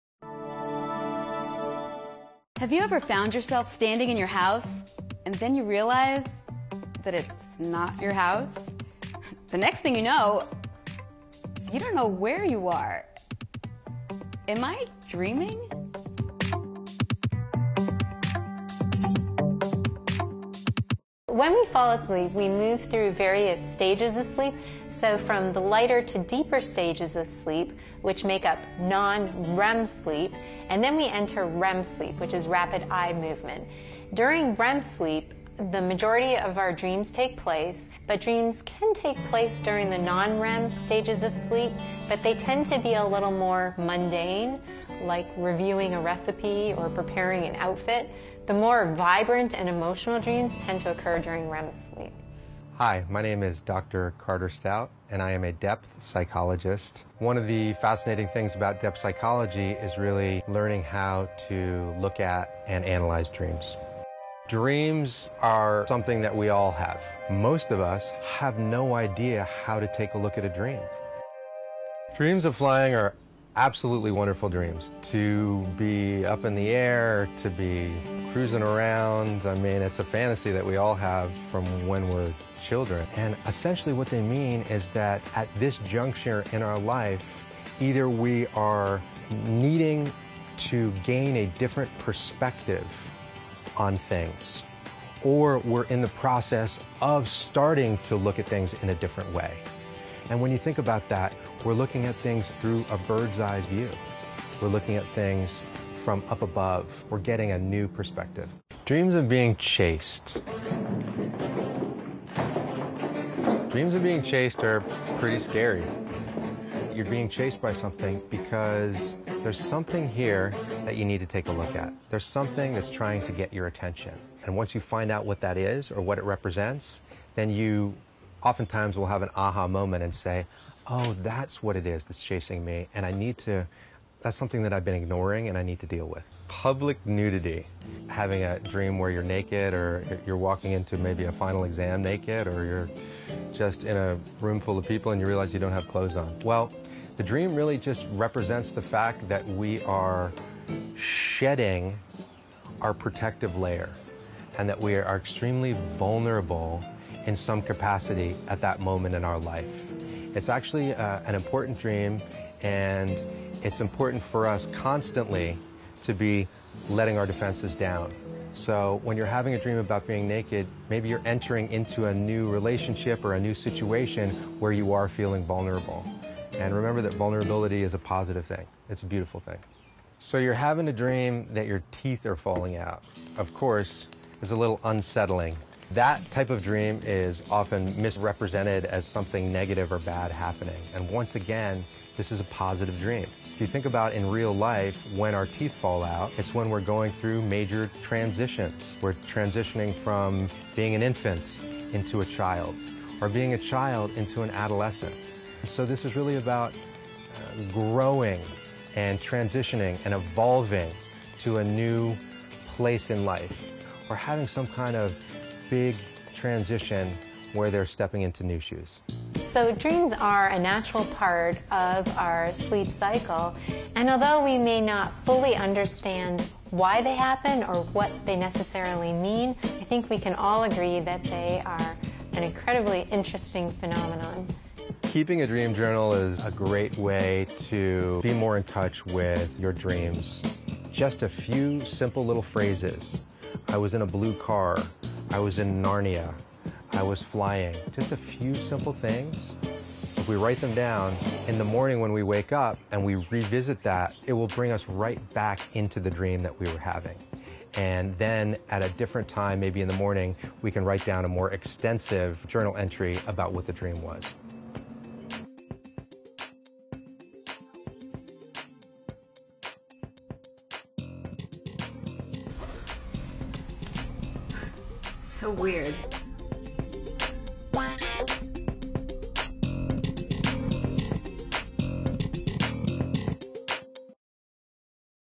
Dream Meanings: What Your Dreams Reveal About You
As you recount a strange dream to a friend, it doesn’t take long to realize how illogical and silly it can sound. The surroundings change without warning, the plot twists faster than a soap opera, and even the people in the dream seamlessly transform into a different characters in bizarre ways. With all this nonsense, you have to wonder—is there true meaning to your dreams? What’s the best way to interpret your dreams? Can certain dream symbols shed light on what’s happening IRL?

According to Natalie D. Dautovich, PhD, of the National Sleep Foundation, dreams occur during the REM cycle of sleep, or Rapid Eye Movement. (Learn more about different sleep cycles and why they matter here.) And yes, if you peek at someone sleeping and see their eyes shifting from side to side, there’s a good chance they’re dreaming—although the dream may not be about anything interesting or memorable. While we like to try to figure out what dreams mean, some dreams are simply mundane activities, like cooking, cleaning, or other everyday tasks.

However, some dreams might benefit from a more thorough dream interpretation, although it’s easy to make the wrong assumptions about what your dreams mean. “Most of us have no idea how to take a look at a dream,” says Carder Stout, PhD, a Los Angeles-based depth psychologist and dream analyst.